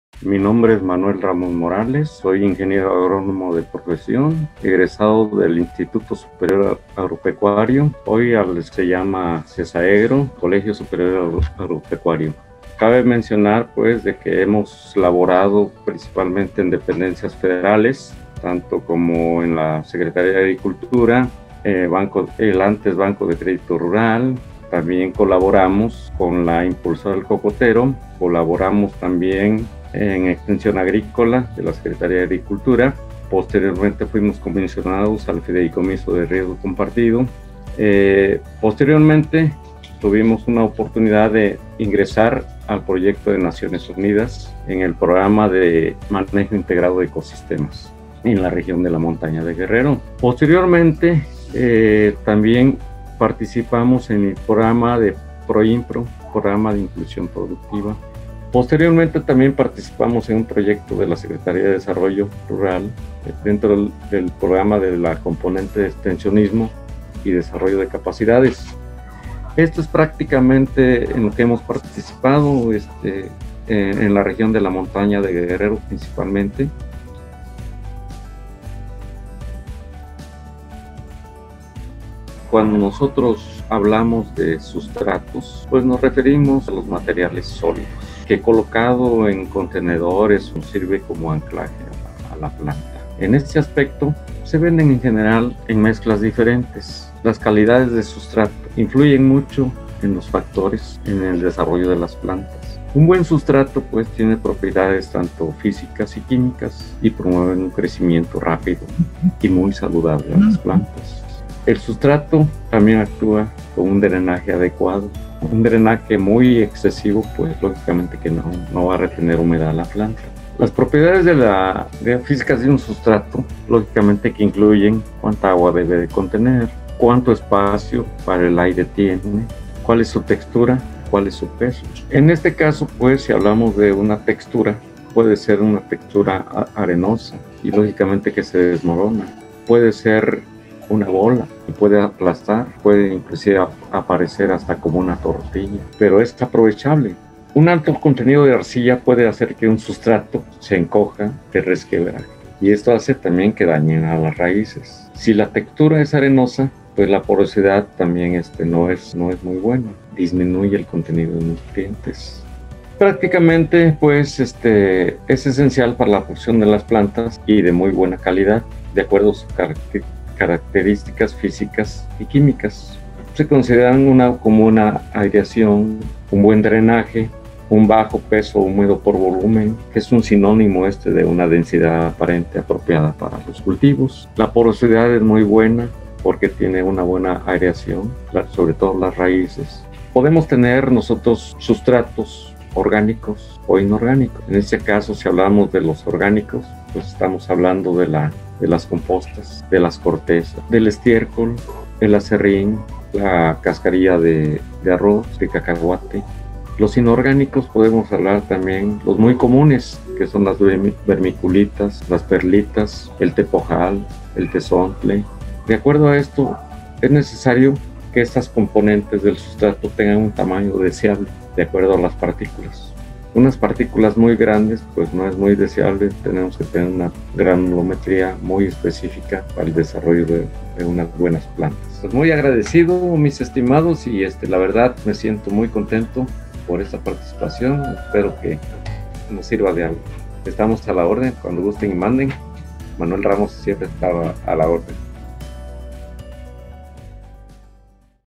Sustratos y su características para la producción agrícola | Ing. Manuel Ramos M.
Si estas por establecer un cultivo y dudas sobre que tipo de sustrato te dará mejores resultados y te facilitará realizar los trabajos propios del cultivo a establecer, dale PLAY al video y conoce directamente del experto en el tema Ing. Manuel Ramos, miembro de la Red Nacional de Capacitadores del sector agroalimentario los tipos de sustratos, las características y funciones de cada uno que te permitirán hacer una elección para optimizar recursos económicos y productivos en tu proyecto.